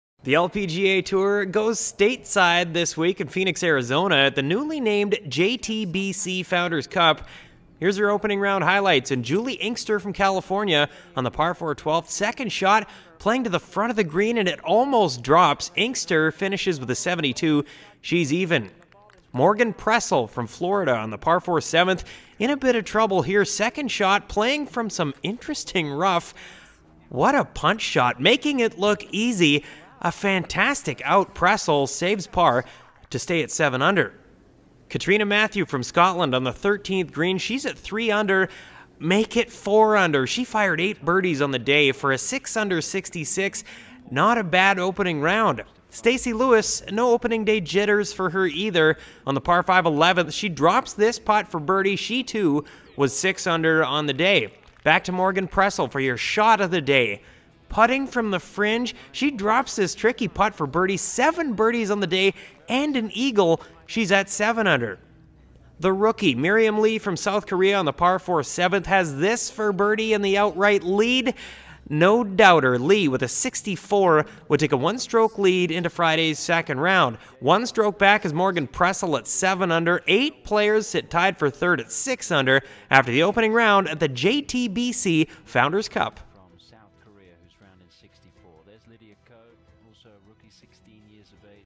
Highlights erste Runde JTBC Founders Cup
Der JTBC Founders Cup ist das erste Turnier der LPGA Tour in der neuen Saison in den USA. Hier gibt es die besten Szenen der ersten Runde aus dem Wildfire Golf Club in Phoenix/Arizona.